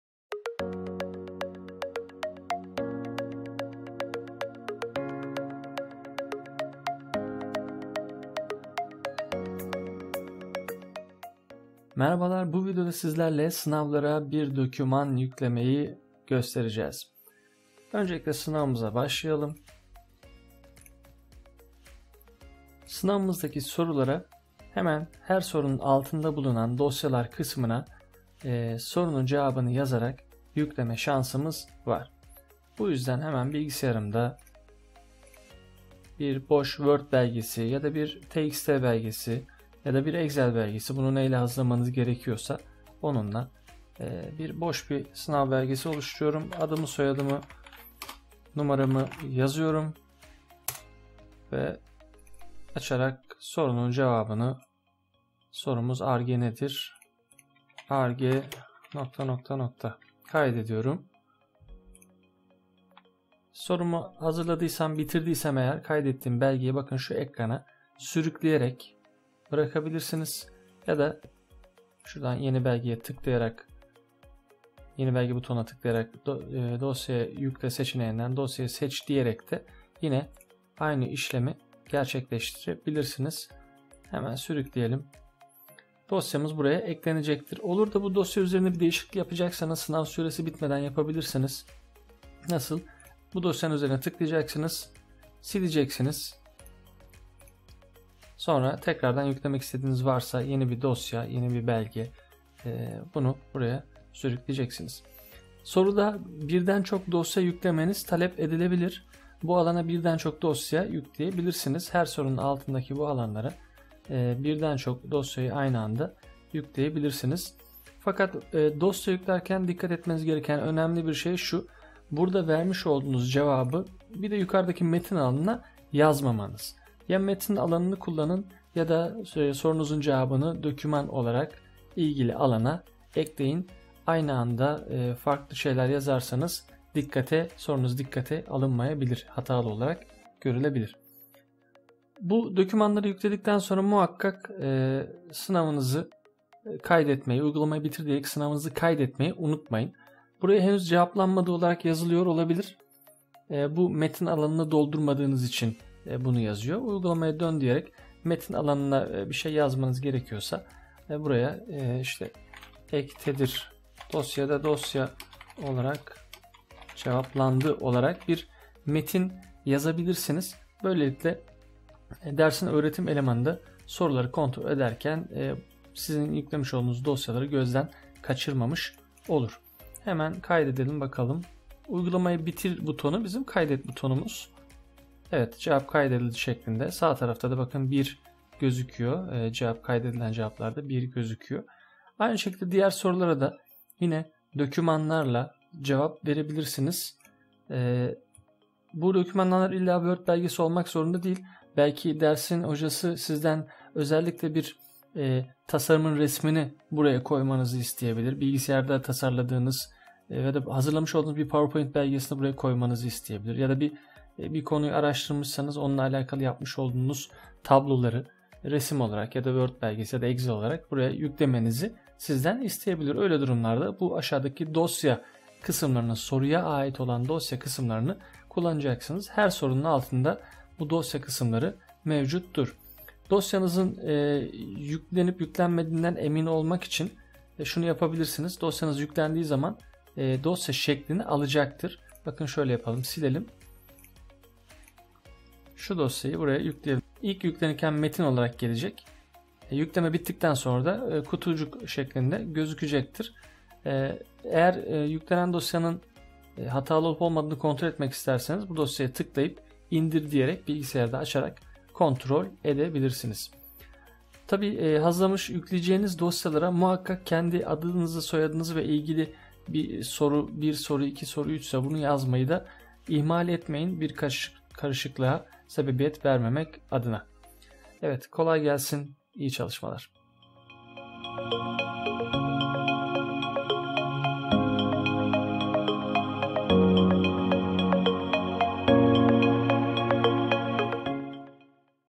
Sorulara dosya yükleyerek cevap verme
Sınav sorularınız farklı şekillerde cevaplar verilebilir. İkinci yöntem, bilgisayarda hazırlayacağınız cevabı, sorunun altında bulunan dosya yükleme alanına yüklemektir. Detaylar videoda.